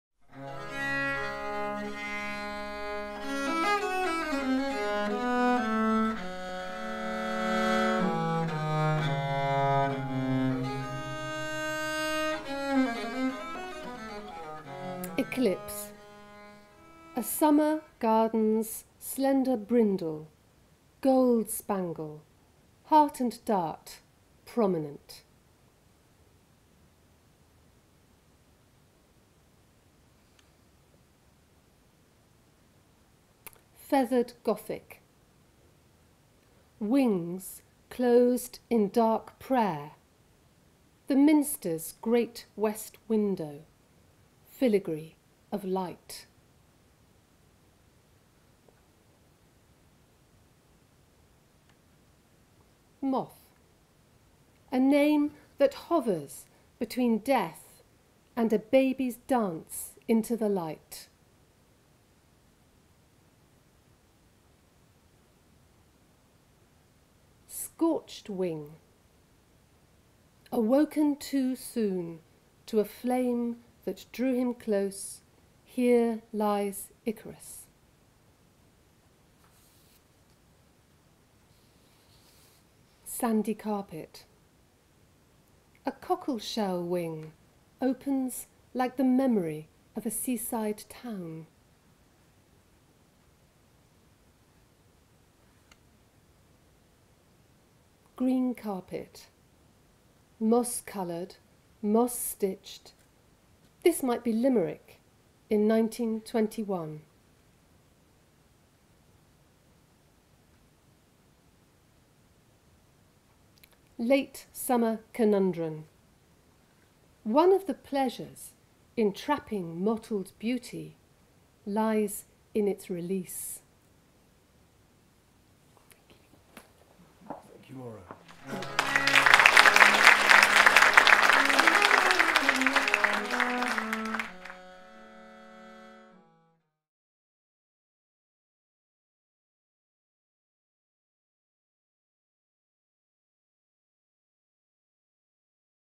Observations/Transformations: A Moth Anthology. Haiku by Maura Dooley.
The Laurence Sterne Trust at Shandy Hall presented 'Observations/Transformations: A Moth Anthology' an evening of sound and image drawing inspiration from the nocturnal world of moths. Friday 20 February 2015 at Rymer Auditorium, Sir Jack Lyons Concert Hall, University of York.

With music from the 18th - 21st century
David Owen Norris - Square Piano
Richard Boothby - Viola da Gamba
Michiyo Takano - Koto
Reading by Professor Frances Spalding
Haiku by Billy Collins, Maura Dooley and Roger Keyes.
Including students from the University of London, Goldsmiths College and Yamanashi Prefectural University, Kofu, Japan

Funded by Arts Council England

For more events and updates from The Laurence Sterne Trust, sign up to our mailing list: